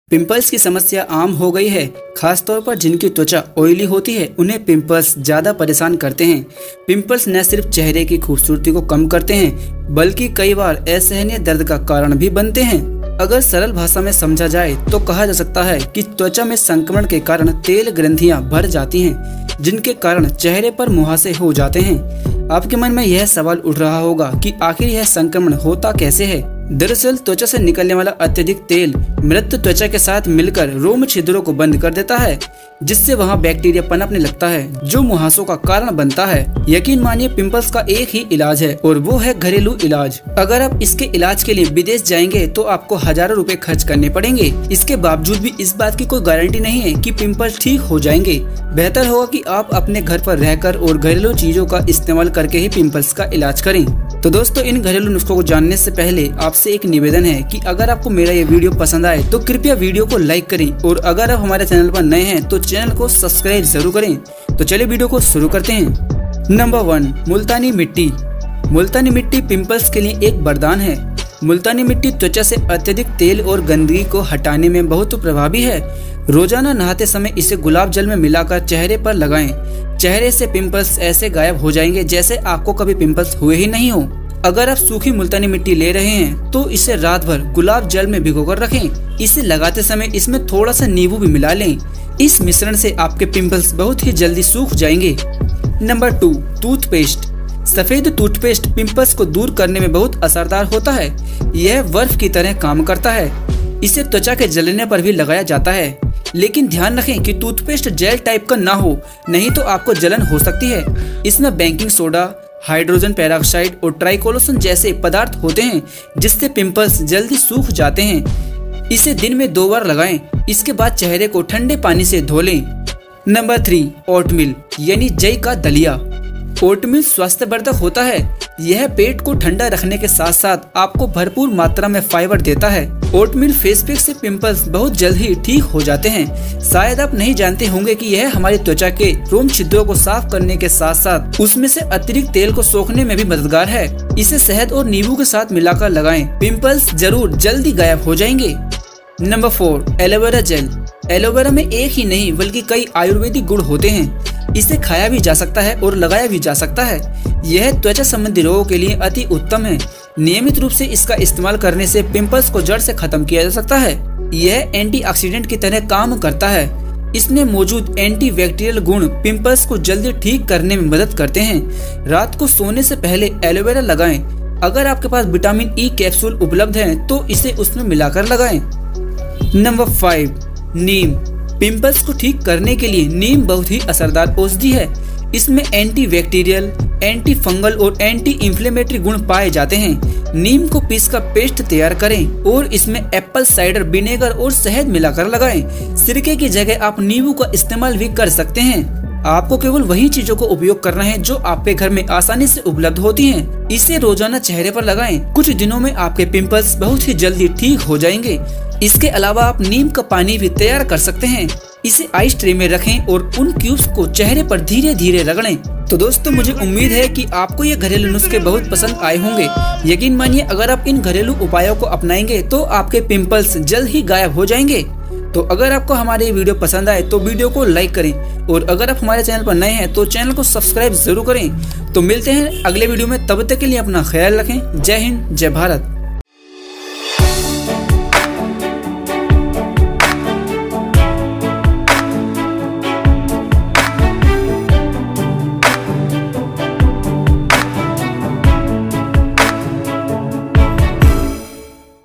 घरेलू नुस्खों से पिम्पल हटाएं, चेहरा चमकाएं | Remove pimples with home remedies, brighten face
~ Channel Info 😅
my name is Saurabh Kumar. On our channel you will get to see the best, fact and knowledge videos.